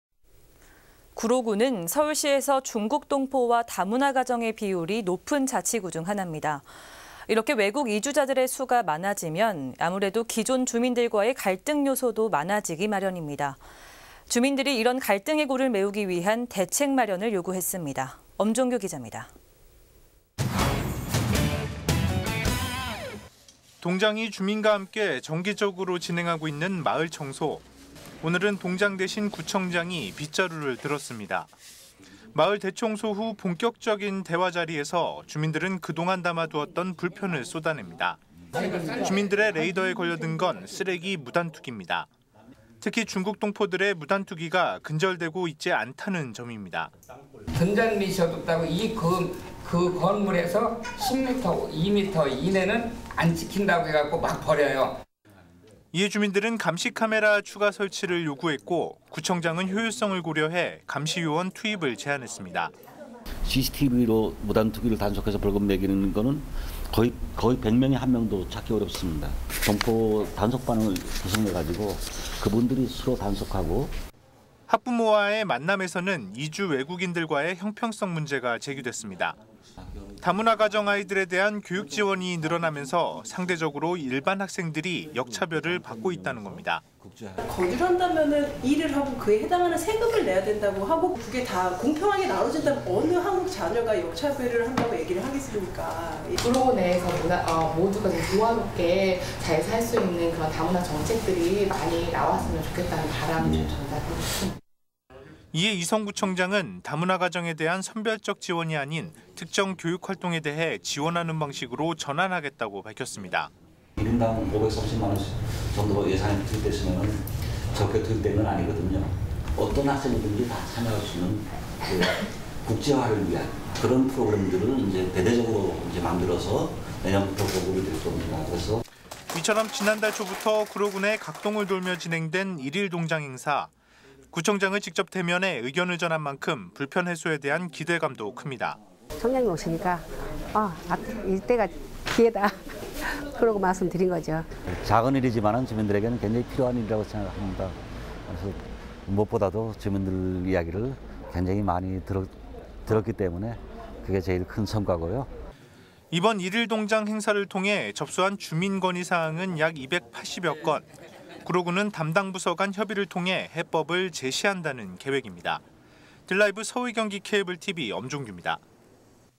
구로_중국 동포와 상생방안 찾는다(서울경기케이블TV뉴스)
【 앵커멘트 】
구로구는 서울시에서
중국동포와 다문화 가정의
비율이 높은 자치구 중
하납니다.
이렇게 외국 이주자들의
수가 많아지면
아무래도
기존 주민들과의 갈등 요소도
많아지기 마련입니다.
주민들이
이런 갈등의 골을 메우기 위한
대책 마련을 요구했습니다.
엄종규 기잡니다

【 기자멘트 】
동장이 주민과 함께 정기적으로
진행하고 있는 마을 청소

오늘은 동장 대신 구청장이 빗자루를
들었습니다.

마을 대청소 후 본격적인 대화 자리에서
주민들은 그동안 담아두었던 불편을
쏟아냅니다.

주민들의 레이더에 걸려든 건
쓰레기 무단 투깁니다.

특히 중국동포들의 무단투기가
근절되고 있지 않다는 점입니다.

【 녹취 】
구자광
위원장/구로2동 바르게살기운동 협의회
등잔 밑이 어둡다고 (CCTV)에서 2m 이내는 안 찍힌다고
(쓰레기를)막 버려요.

【 기자멘트 】
이에 주민들은 감시카메라 추가 설치를
요구했고 구청장은 효율성을 고려해
감시요원 투입을 제안했습니다.

【 녹취 】
이성
구로구청장
CCTV로 무단투기를 단속해서 벌금 매기는 건 거의 100명에
한명도 찾기 어렵습니다. 동포 단속반을 구성해서

이성
구로구청장
그분들이 스스로 단속하고

【 기자멘트 】
학부모와의 만남에서도
이주외국인들과의 형평성
문제가 제기됐습니다.

다문화 가정 아이들에 대한
교육지원이 늘어나면서 일반
학생들이 역차별을 받고 있다는
겁니다.

【 녹취 】
박정숙
구로구 구로2동
(동포들이 구로에)거주를 한다면 일을 하고 그에 해당하는 세금을
내야하고 그게 다 공평하게 나눠진다면 어느 한국 자녀가 역차별을

박정숙
한다고 얘기하겠습니까?

【 녹취 】
최은선
구로 내에서 모두가 조화롭게 잘살 수 있는 다문화 정책들이
많이 나왔으면 좋겠다는 바람을 전하고 싶습니다.

【 기자멘트 】
이에 이성구청장은 다문화 가정에 대한
선별적 지원이 아닌 특정 교육활동에 대한
지원하는 방식으로 전환하겠다고 밝혔습니다.

【 녹취 】
이성
구로구청장
(다문화 학생)1인당 530만원 씩 예산이 투입됐으면 적게
투입된 건 아니거든요 어떤 학생이든지 다 참여할 수 있는

이성
구로구청장
국제화를 위한 프로그램을 대대적으로 만들어
내년부터 보급이 될 겁니다.

【 기자멘트 】
이처럼 지난달 초부터 구로구 내
각 동을 돌며 진행된 일일동장 행사

구청장을 직접 대면해 의견을 전한 만큼
불편 해소에 대한 기대도 큽니다.

【 인터뷰 】
강순희
회장 / 구로2동 통장친목회

【 인터뷰 】
이성
구로구청장

【 기자멘트 】
이번 일일동장 행사를 통해
접수한 주민 건의사항은 약
280여건

구로구는 담당부서 간 협의를 통해
해법을 제시한다는 계획입니다.

딜라이브 서울경기케이블TV 엄종귭니다.